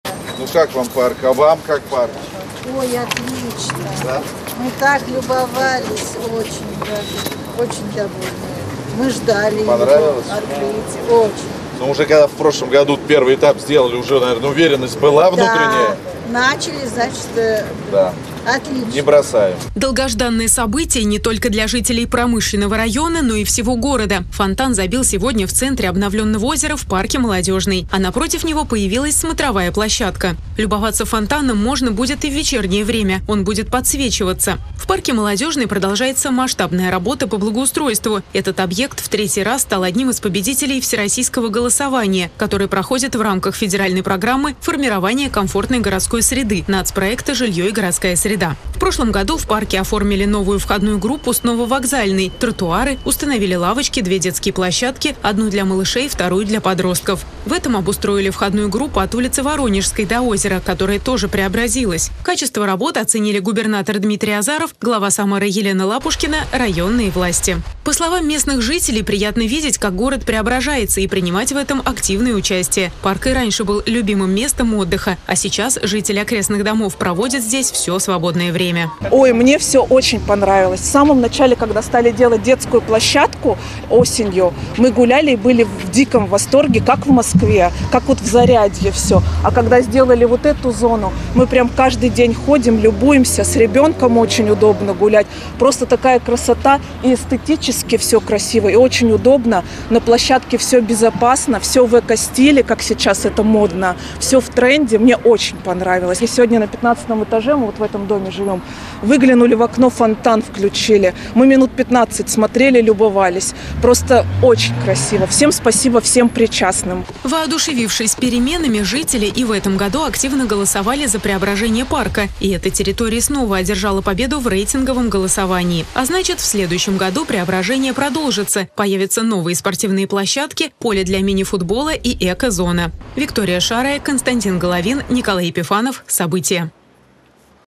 Губернатор Дмитрий Азаров проинспектировал обновленный парк "Молодежный" в Самаре
Событие, которого ждали все лето. В центре озера, расположенного в парке "Молодежный", забил фонтан с подсветкой. А напротив него появилась смотровая площадка. В парке Молодежный продолжается масштабная работа по благоустройству. Качество работ оценили губернатор Дмитрий Азаров, глава Самары Елена Лапушкина, районные власти.